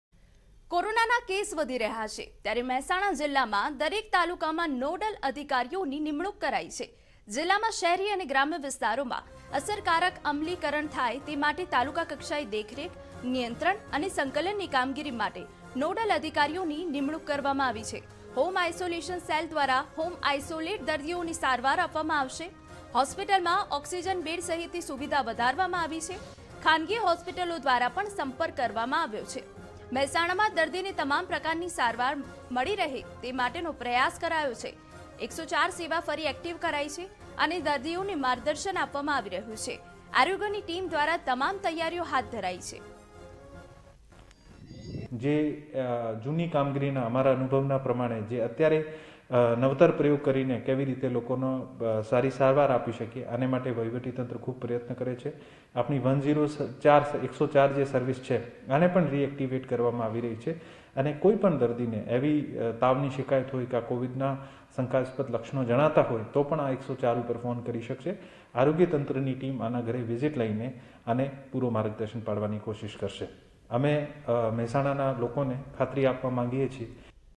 Mehsana: Nodal officer appointed for handling COVID19 Public grievances| TV9News
Mehsana: Nodal officer appointed for handling COVID19 Public grievances| N06